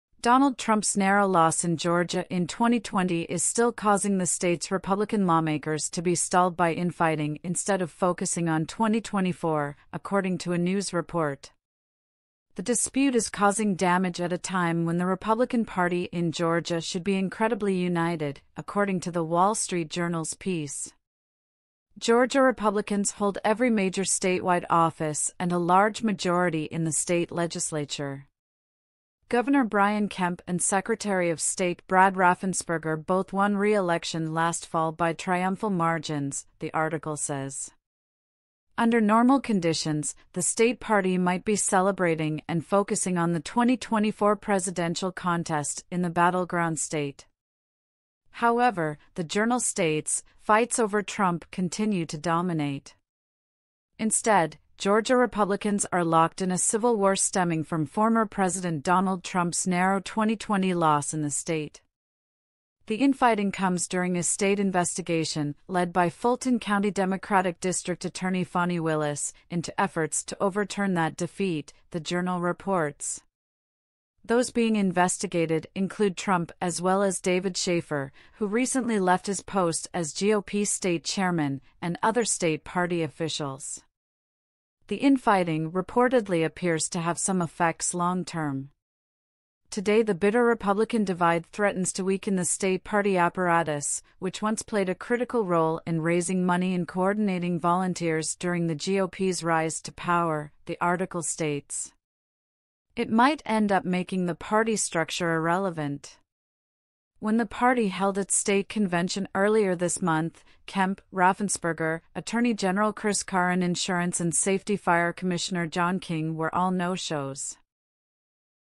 Trump's Georgia loss still causing state GOP officials to be 'locked in a civil war'
Donald Trump's tight misfortune in Georgia in 2020 is as yet making the state's conservative legislators be slowed down by infighting as opposed to zeroing in on 2024, as per a news report.